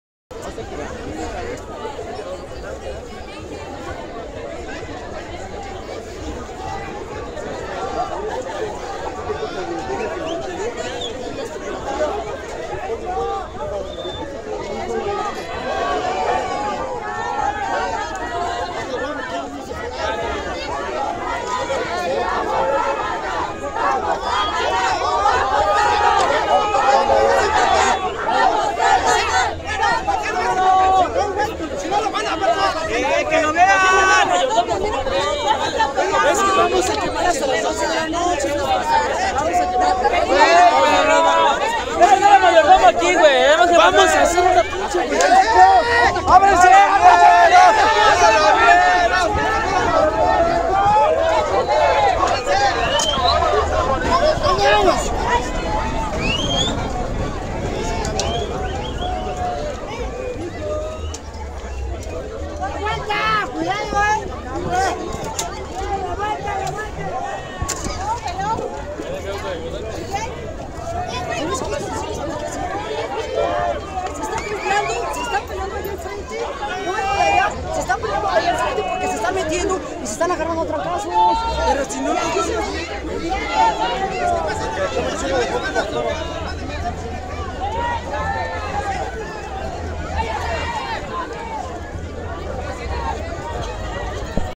Recorrido de Toros Pirotécnicos durante la Feria Nacional de la Pirotecnia Tultepec 2014 (6)
Tultepec, Méx. Recorrido de "toros" que participarán en la Quema de Toros Pirotécnicos durante la Feria Nacional de la Pirotecnia Tultepec 2014, que se realizó en la plaza principal de la población este sábado 8 de marzo. Video 6 (de 7).